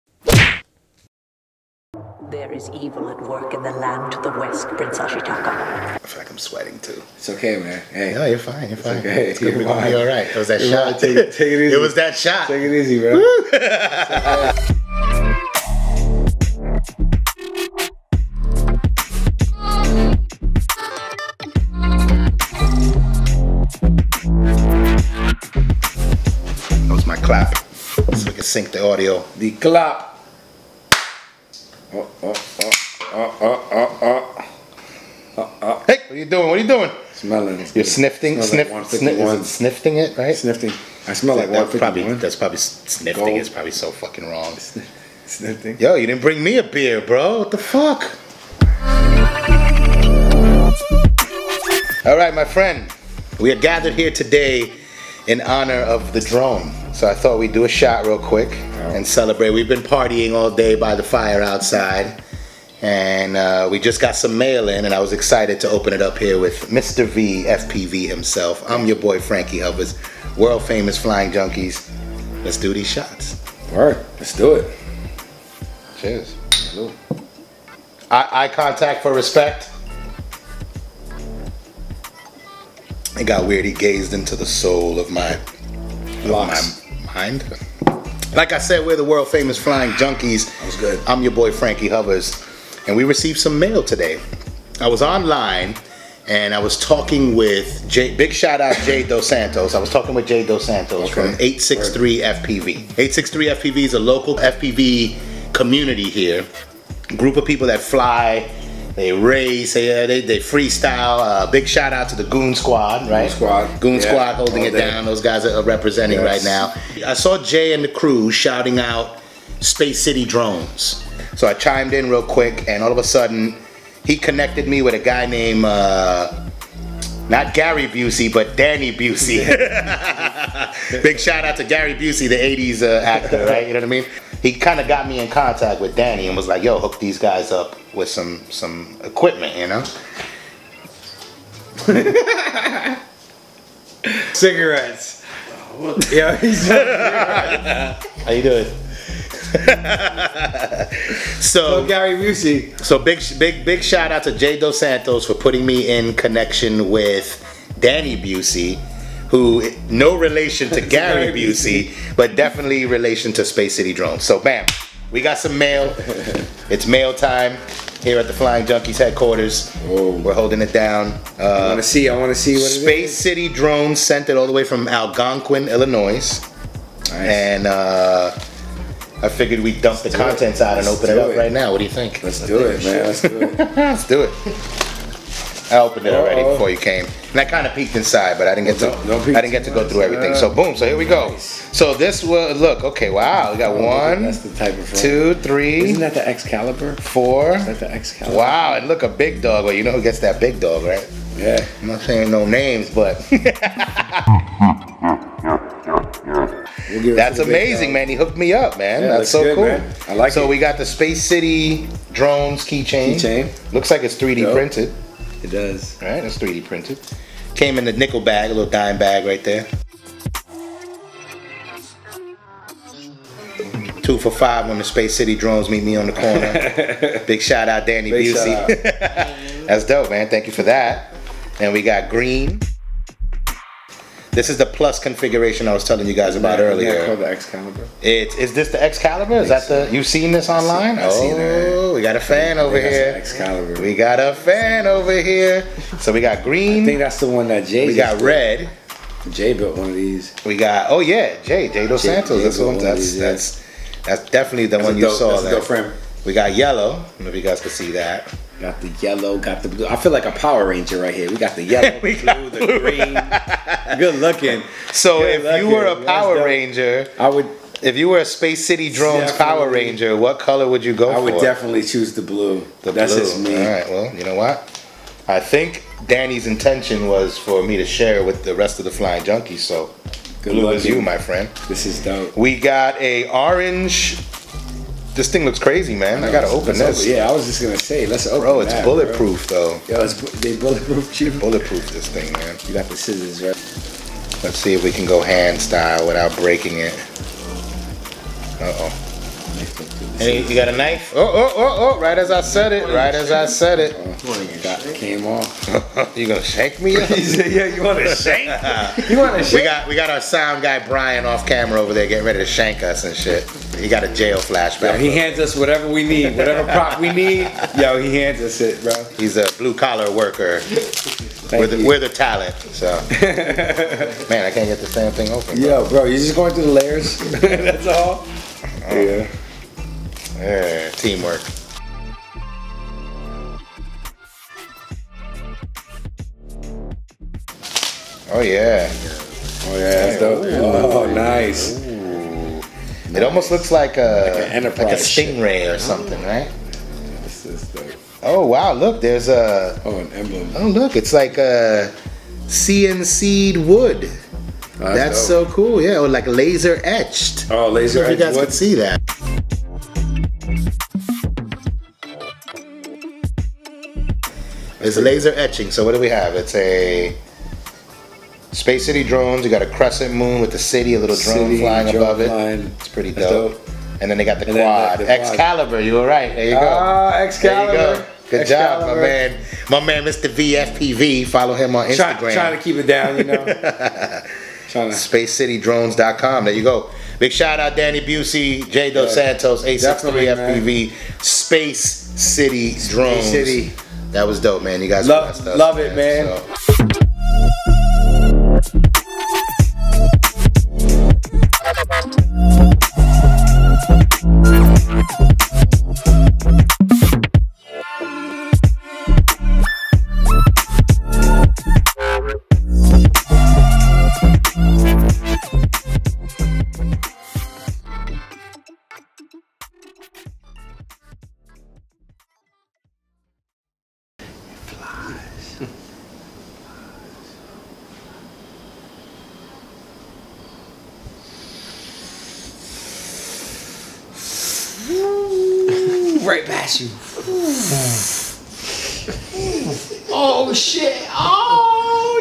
3D Printed Keychain Space City Drones
3D Printed Keychains by Space City Drones

These Keychains incorporate elements of 3D Printing & Laser Engraving while brining awareness to the company's flagship product the "Excalibur" a carbon fiber airframe which breaks from the traditional "X" shape in favor of a Plus "+" shape.

a Special thank you to 863FPV, Jay DosSantos & the Goonskwad for introducing us to Danny Beusse of Space City Drones